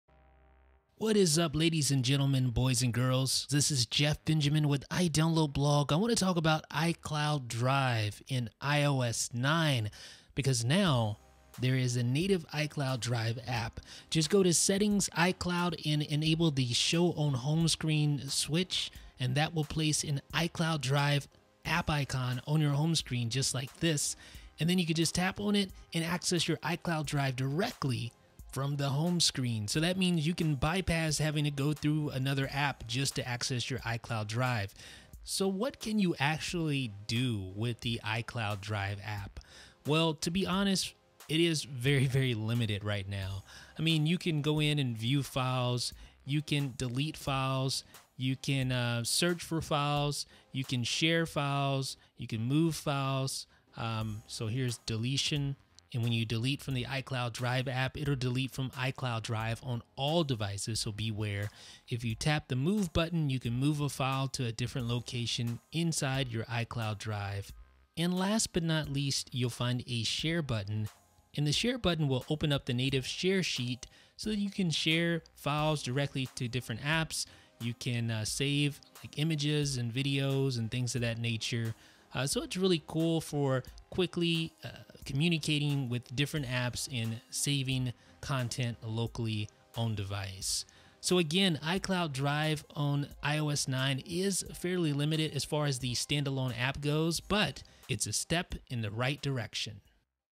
How to enable and use the iCloud Drive app in iOS 9
A tutorial that shows how to enable iCloud Drive and how to use its limited feature-set.

About iDB